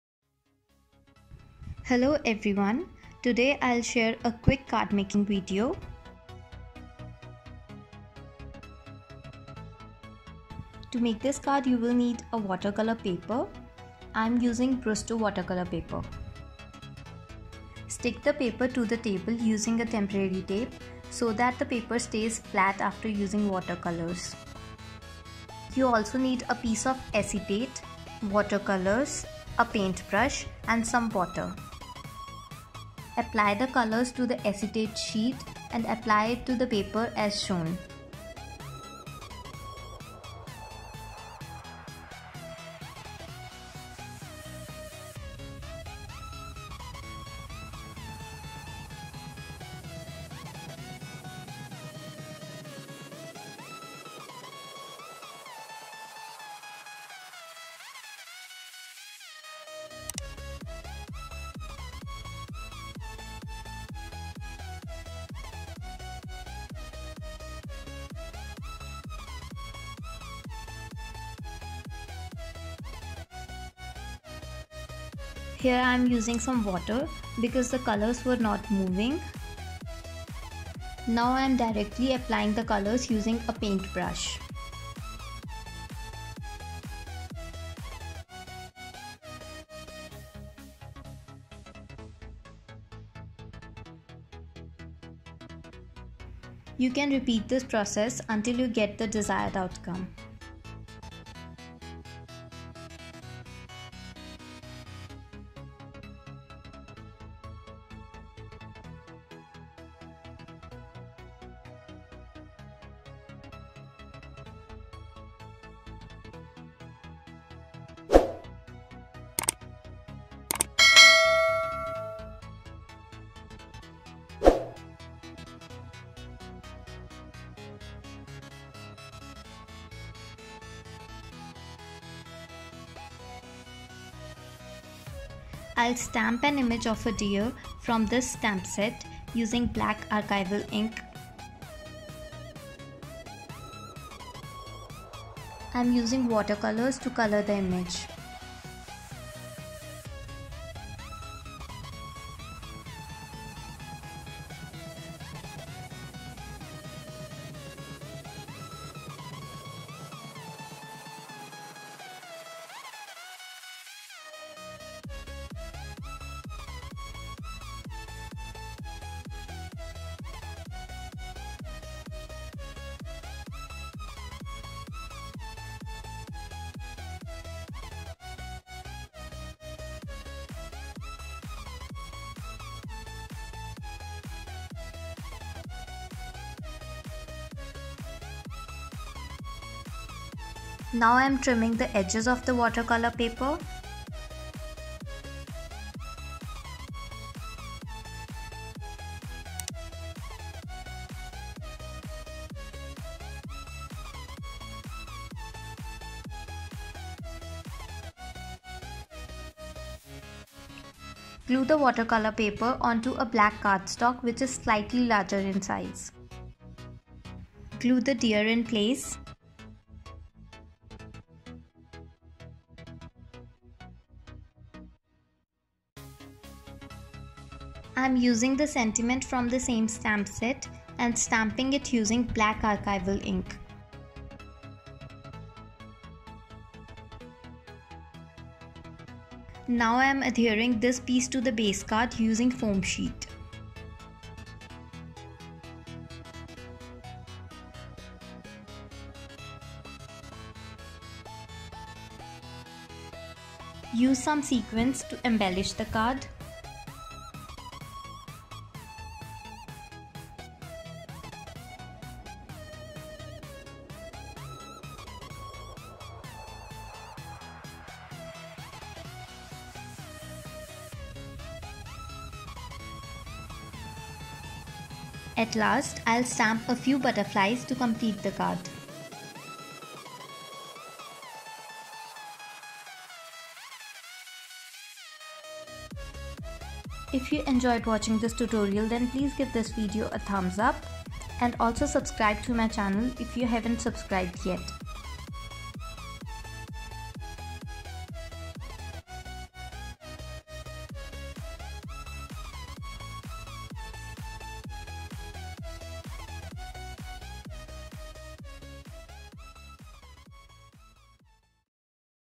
Quick Card Making Tutorial | Praveen's Crafty Corner
Hello craft buddies!
In this video, I'll share will you all a quick card making video.  If you don't have the products I used in this tutorial then you can definitely use anything that you have with you. If you get inspired to create a card after watching this video then do share your creations with me. I would love to see your creations.

Here's the list of all the craft supplies that I used in this tutorial:
1. Brusto Artists' Watercolor Paper
4. Water Colors
5. Ranger Archival Ink Pad - Jet Black: shorturl.at/gryHN

Thank you for taking out the time to watch my video. It really means a lot to me. Hope you have a great day!